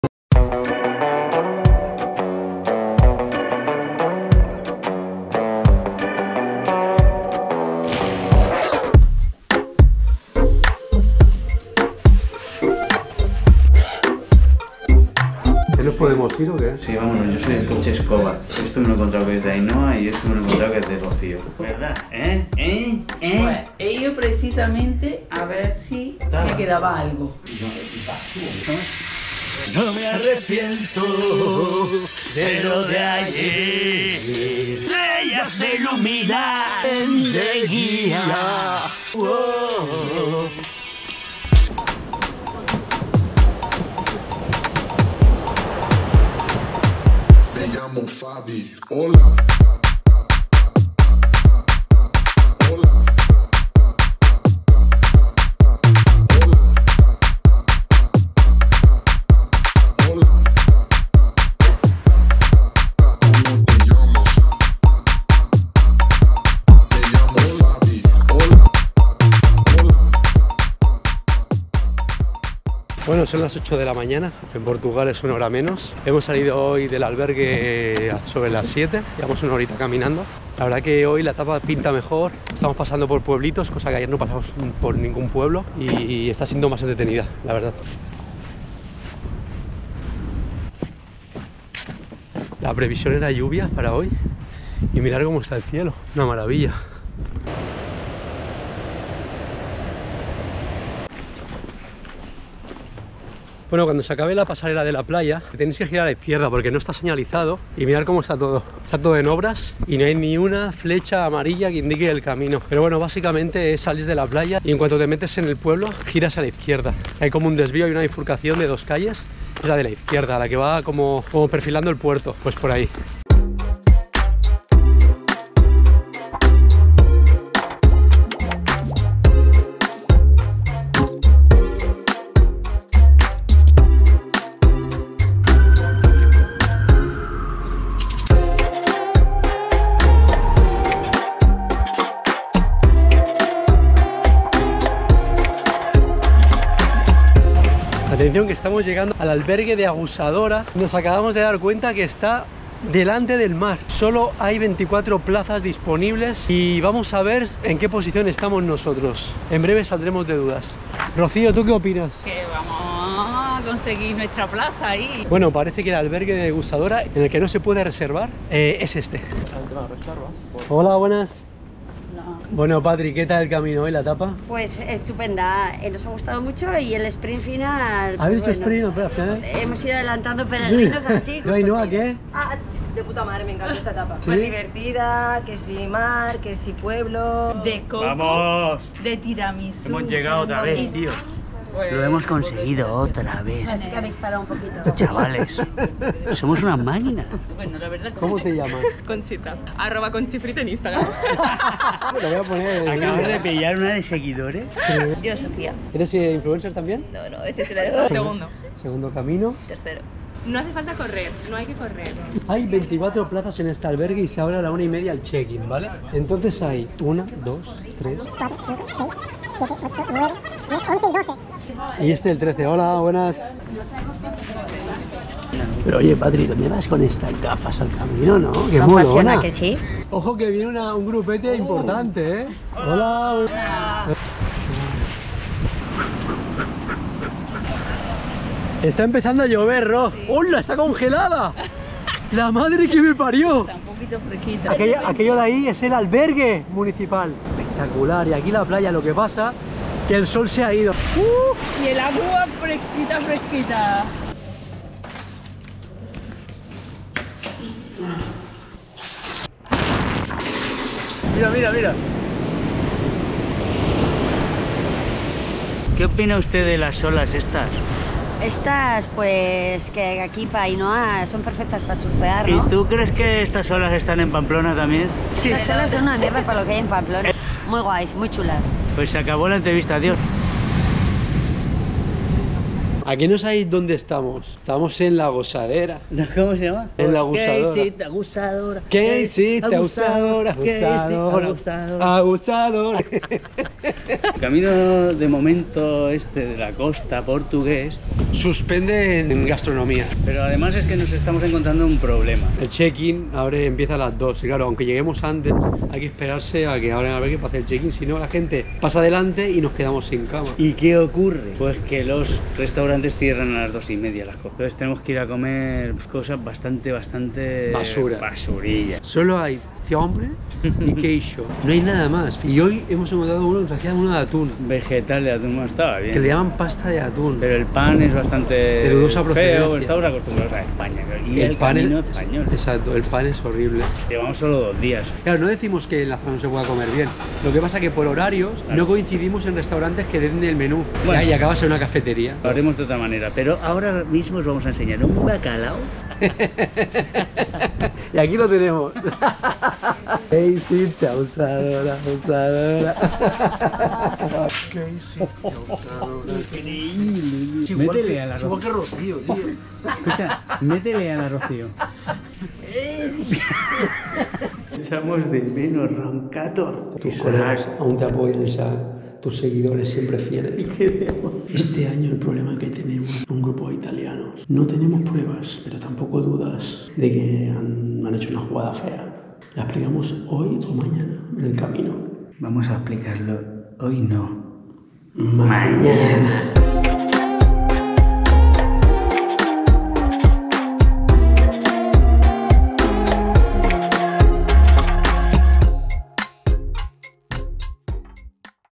CAMINO PORTUGUES - ETAPA 2
El CAMINO PORTUGUES DE LA COSTA sigue el litoral atlántico desde OPORTO, pasando por Viana do Castelo, A Guarda y Vigo, hasta unirse al Camino Portugués Central en Redondela.